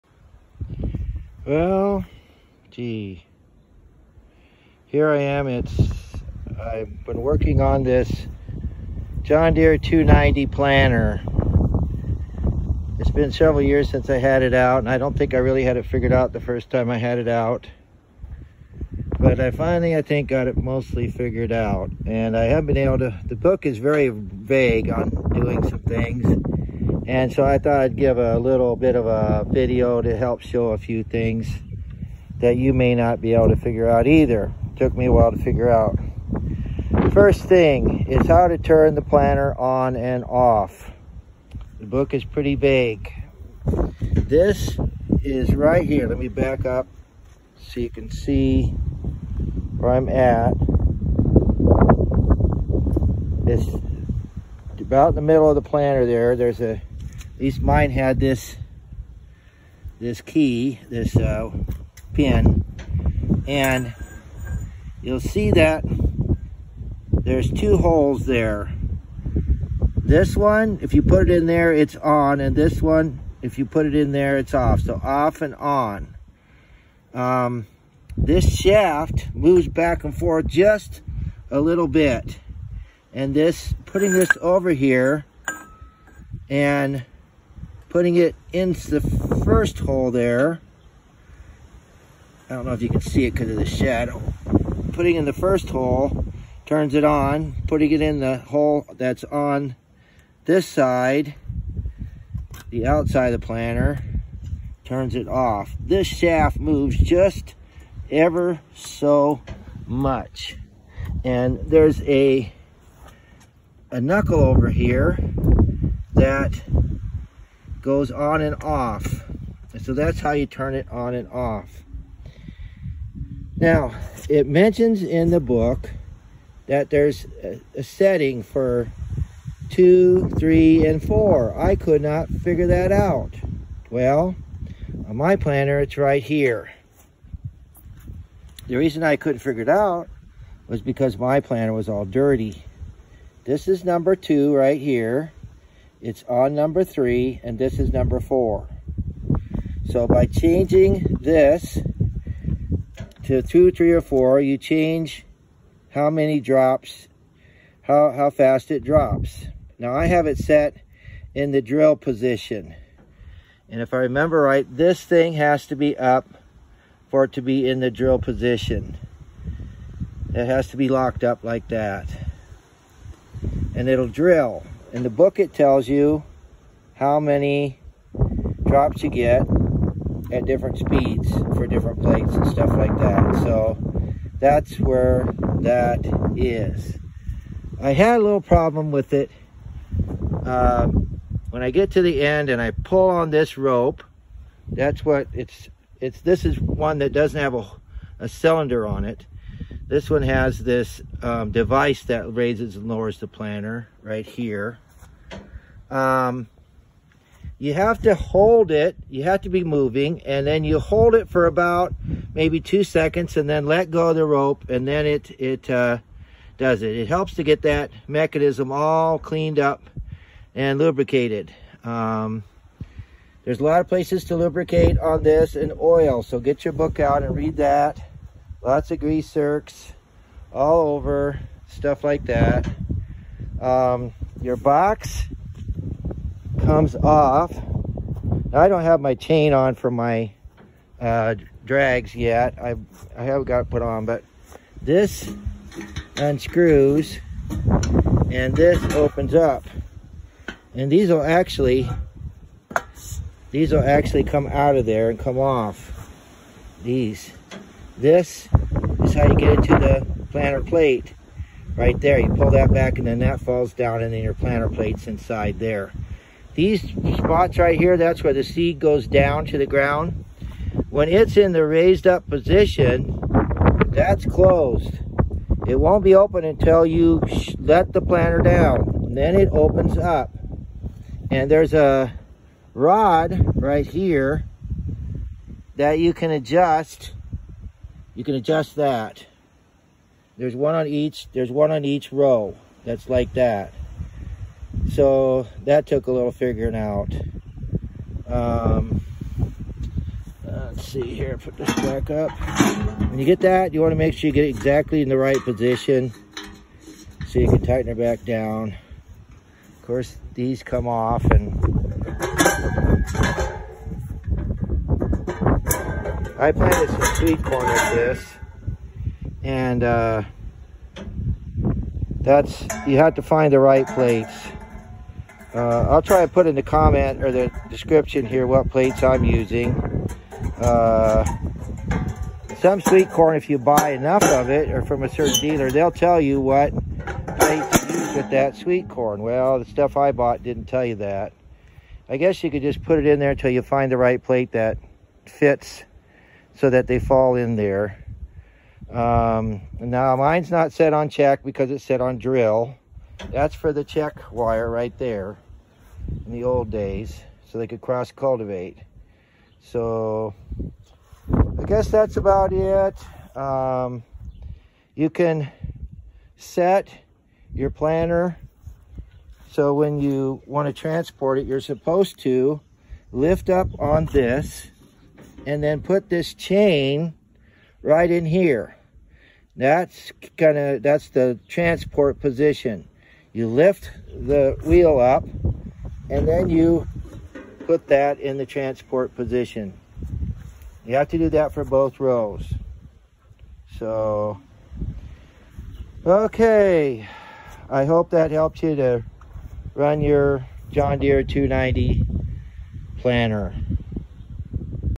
Operation of a John Deere 290 two row corn planter. Some help I hope
How to operate a Old John Deere 290 two row planter. The operators book is vague. So I hope this is helpful to you. One plate we used on some of the newer smaller sweet corn was H1268-B   or B4.  Thanks